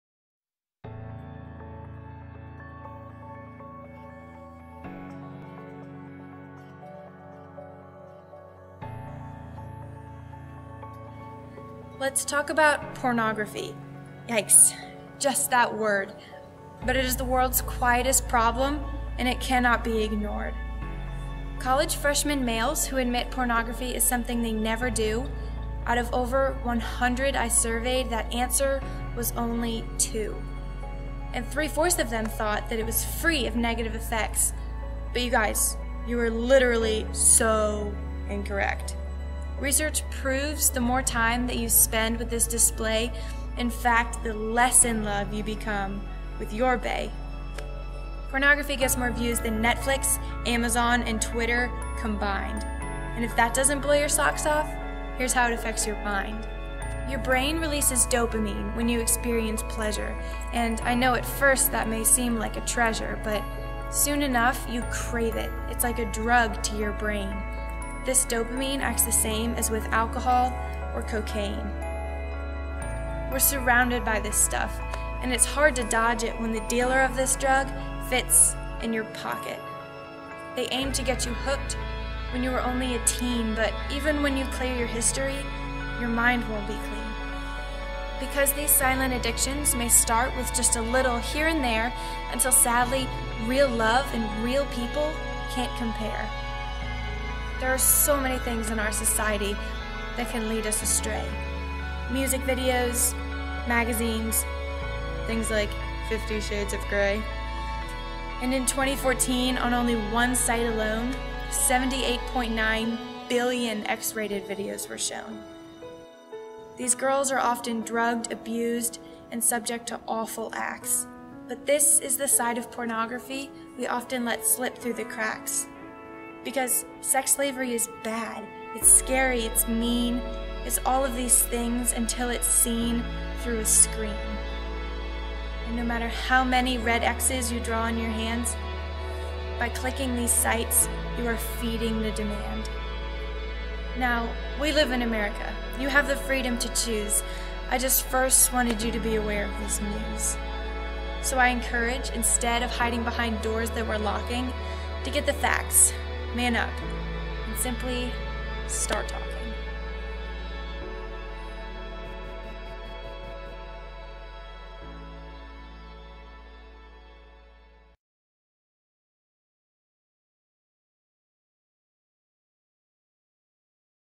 Start Talking
Alexa Connelly
GWRTC 103 - Section 45

This is a PSA/spoken word type of video about the harmful effects pornography has on your brain, your relationships, and society!